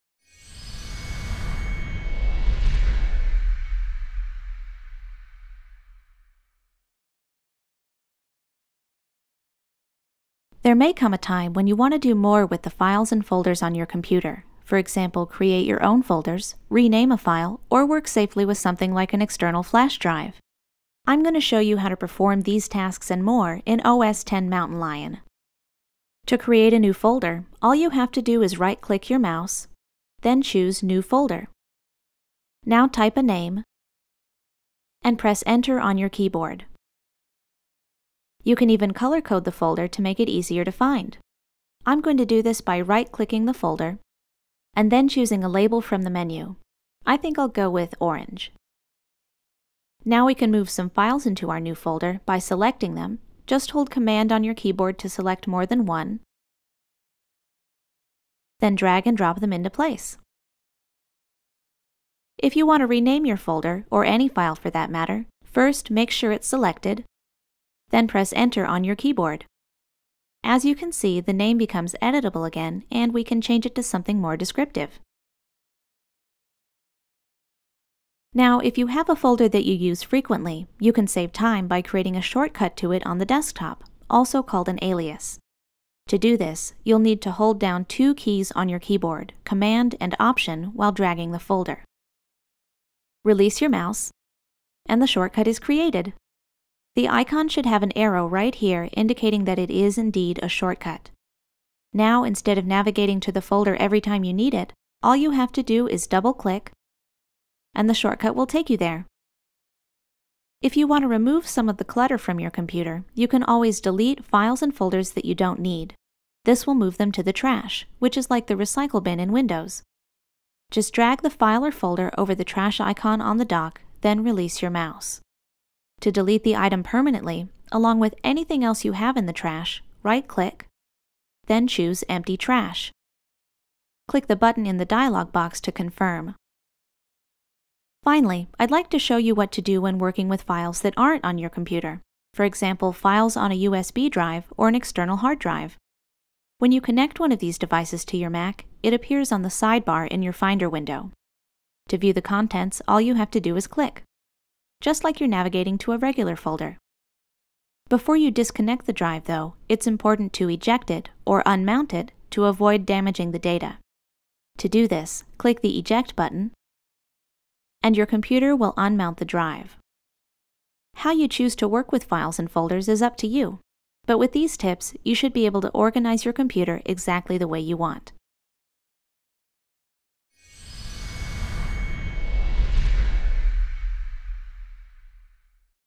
Mac OS X Mountain Lion: Working with Files and Folders
In this video, you’ll learn more about working with files and folders in Mac OS X Mountain Lion. for our text-based lesson.

This video includes information on:
• Creating and modifying files and folders
• Creating a shortcut to a folder
• Deleting files and folders